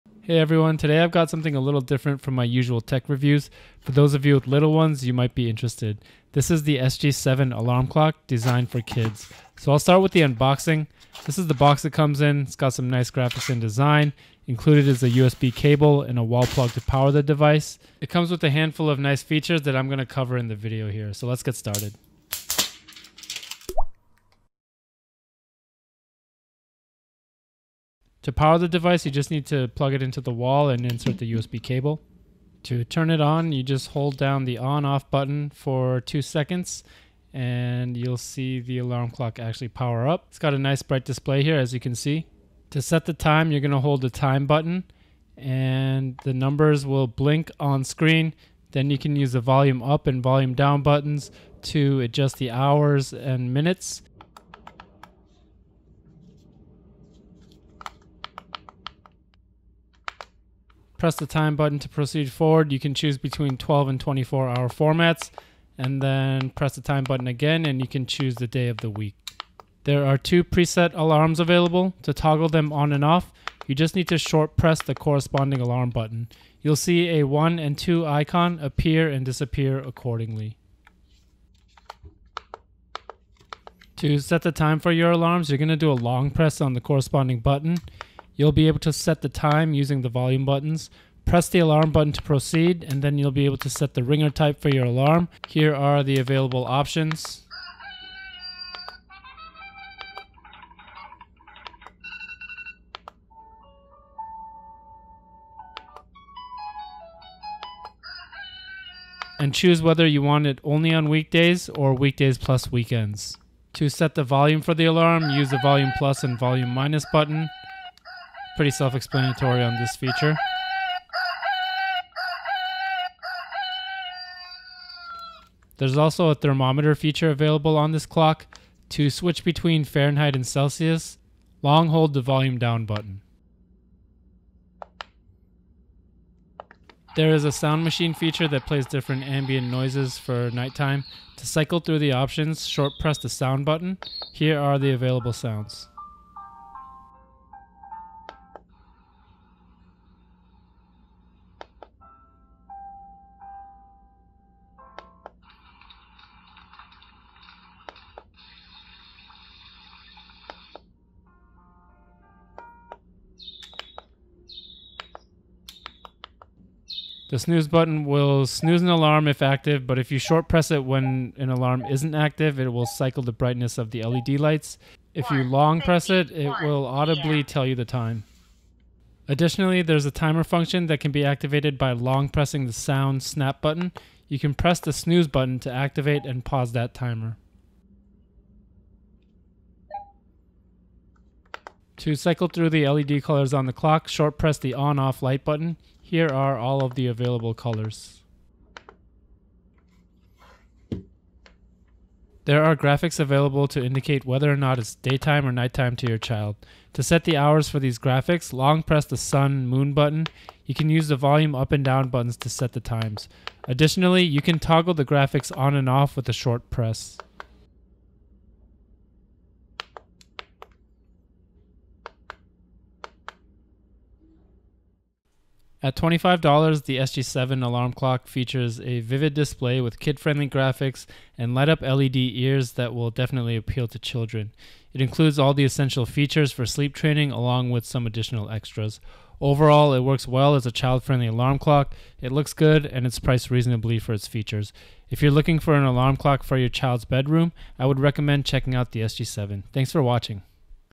Cute Kids' Alarm Clock Review: Packed with Features!
VIDEO CONTENTS
00:00 Unboxing
00:26 Turning it on
00:42 Setting the time
01:09 Alarm function
01:52 Setting volume
02:04 Thermometer
02:15 Sounds
02:45 Snooze button
02:57 Timer function
03:10 LED lights
03:20 Daytime/Nighttime graphics
03:43 Final thoughts